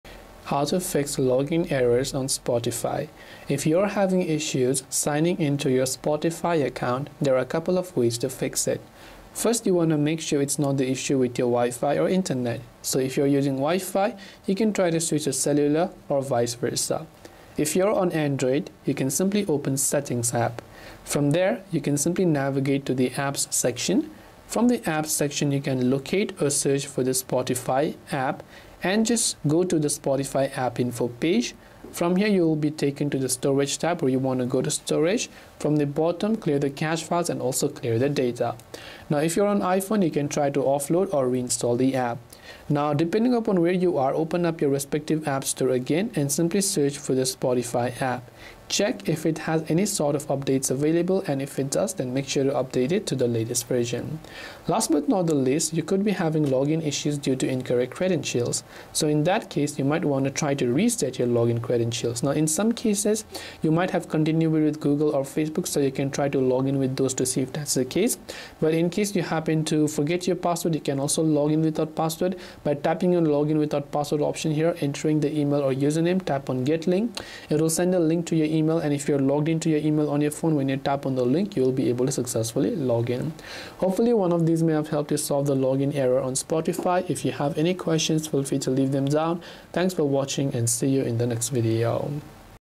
How to Fix Login Errors on Spotify !
Fixing login errors on the Spotify app involves addressing common issues like internet connectivity, outdated app versions, or incorrect login credentials. Follow these steps to troubleshoot and resolve the issue effectively.

First, check your internet connection. Ensure you have a stable connection by switching between Wi-Fi and cellular data to rule out network-related problems.

If the issue persists, clear the app’s cache and data. For Android users, open your device’s settings, navigate to the apps section, and search for Spotify. Select the app and go to its storage options. Clear both the cache and data to reset the app. For iPhone users, use the offload option in your device’s settings or reinstall the app from the App Store to remove temporary files and refresh the app.

Next, ensure the app is updated to the latest version. Open the Google Play Store or Apple App Store, search for Spotify, and check if an update is available. If there is one, update the app to ensure you have the latest bug fixes and performance improvements.

If login issues persist, verify your login credentials. If you’ve forgotten your password, use the "Forgot Password" option on the login screen. Spotify will send a password reset link to your registered email. Follow the link to create a new password and regain access to your account.

If your account is linked to Google or Facebook, try logging in with those credentials. Select the respective login option and follow the prompts. If you’re unable to remember your password, Spotify also offers a "Login Without Password" option. Enter your email or username, tap "Get Link," and check your email for a login link. Clicking this link will log you into your account without requiring a password.

By following these steps, you should be able to resolve most login errors on Spotify. If none of these solutions work, contact Spotify support for further assistance.

Timestamps:
0:00 Introduction
0:07 Checking Internet Connection
0:19 Clearing Cache and Data on Android
0:39 Offloading or Reinstalling the App on iPhone
0:46 Updating the Spotify App
0:54 Verifying Login Credentials
1:06 Using Google or Facebook for Login
1:13 Logging in Without a Password
1:24 Conclusion